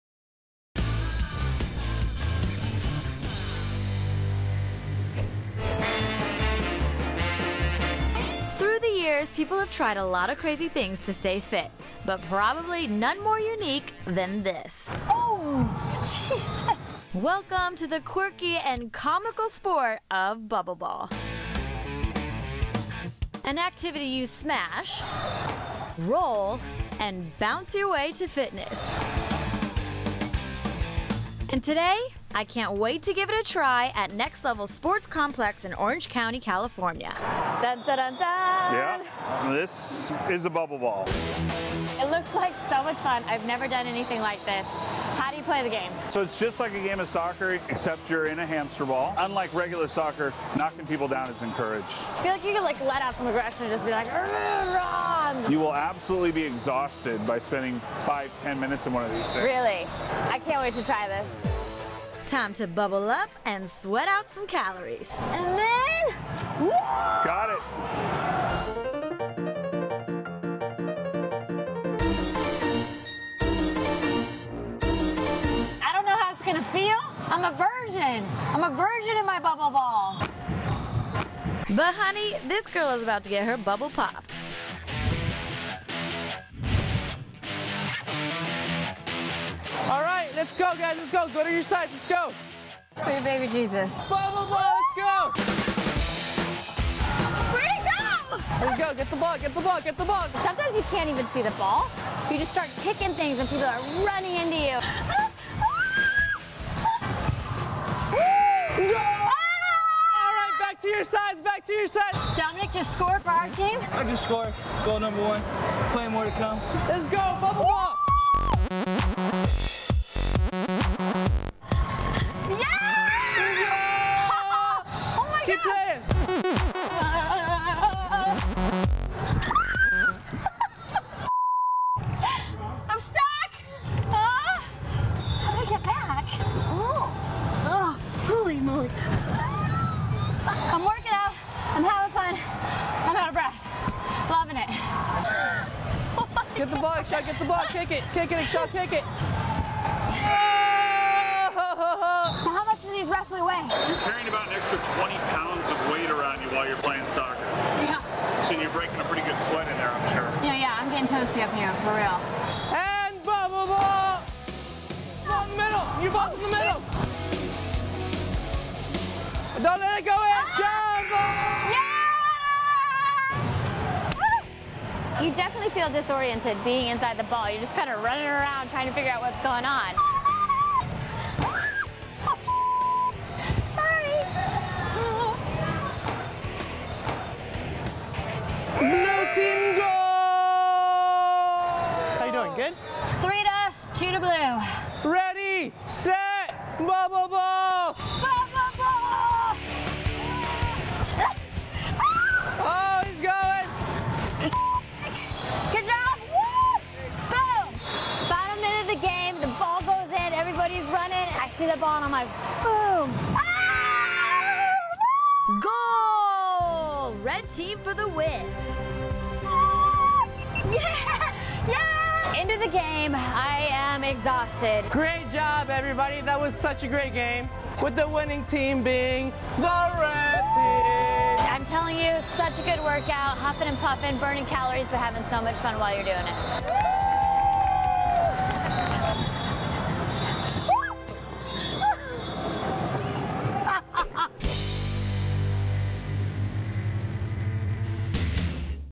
Playing Inside a Giant Inflatable Hamster Ball
Ashley tries out Bubble Ball -- a team activity where players race across a field battling their opponents to score goals-- all while inside a giant inflatable bubble suit.